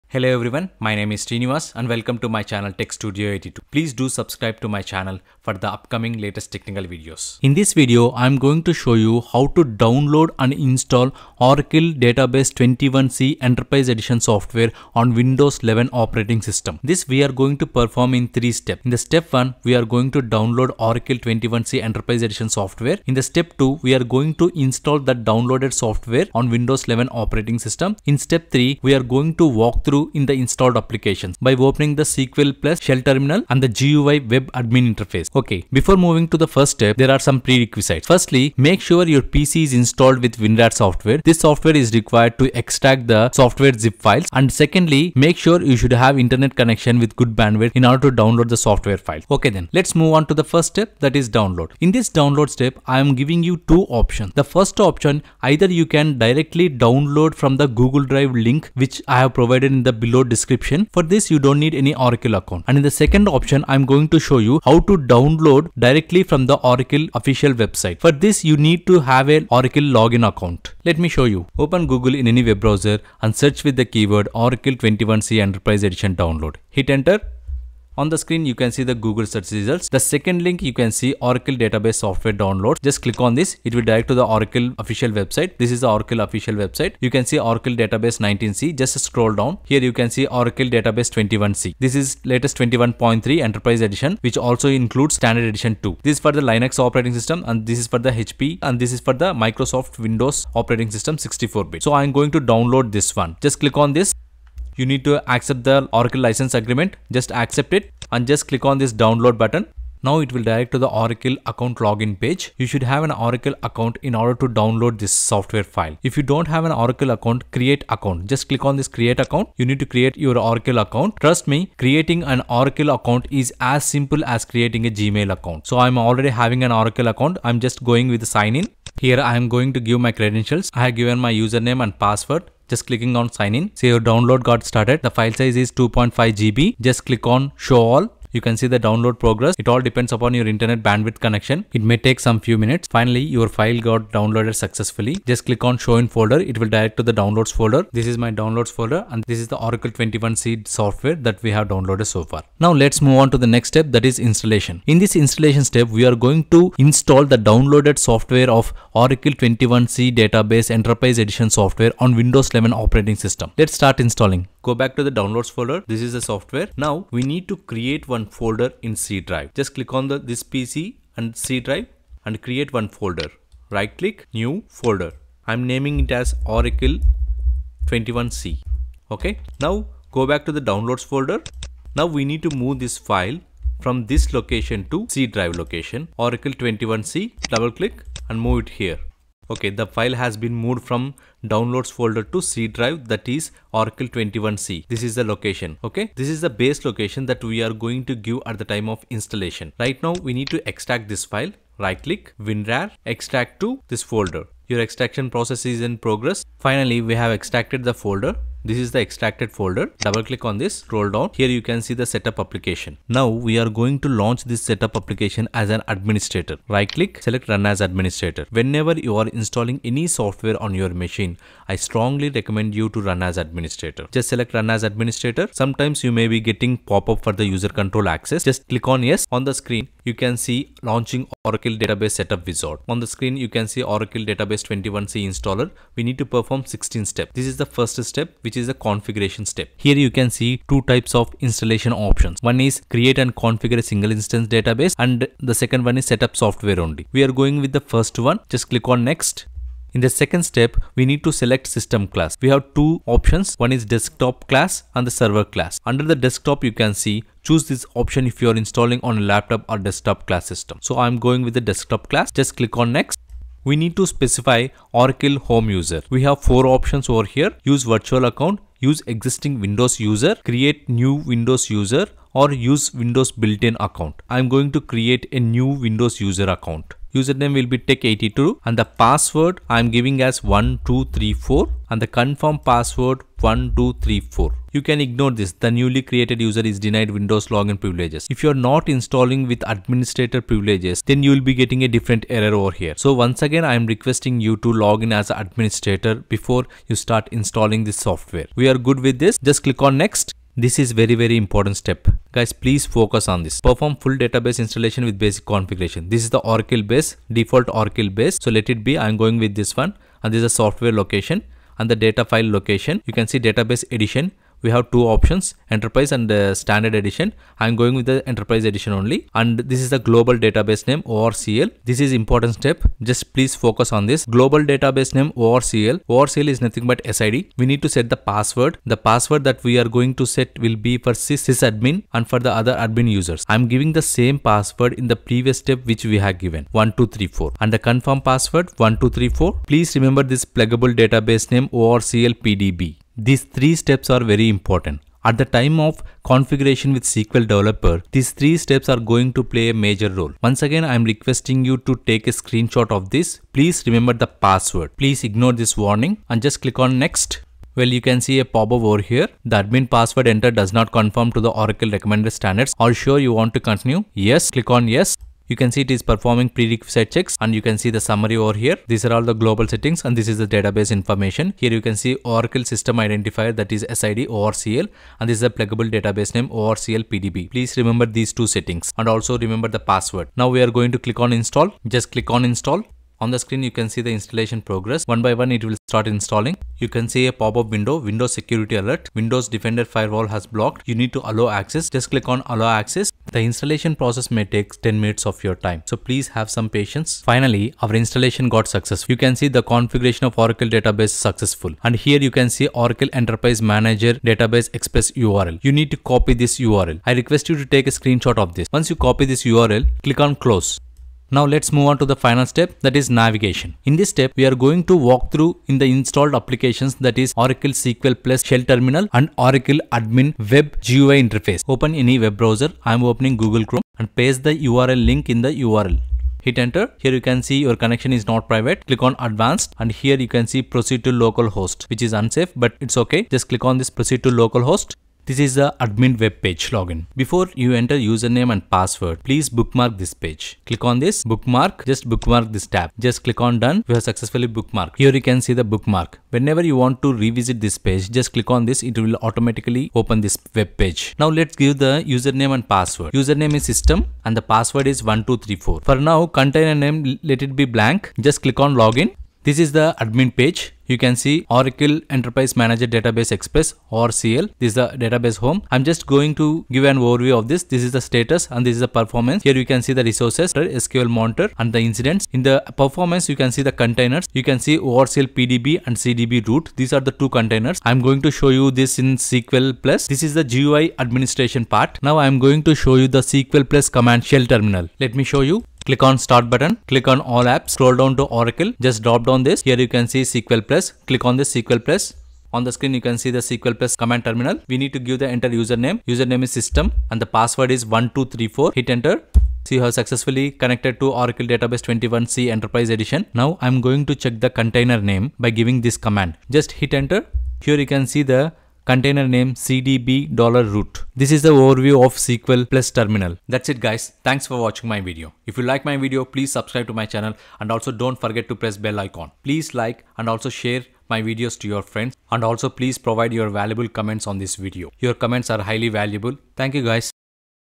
Oracle Database 21c Installation on Windows 11 - Enterprise Edition - Download and Install
Hello Viewers,

Please check the below information,

You can Download directly from my google drive link for Oracle 21c Enterprise  Edition

Please watch the video carefully and install Oracle 21c Enterprise Edition on your machine.

If you encountered with any errors, please share the errors with me in the below comments Section.

Thanks,
TechStudio82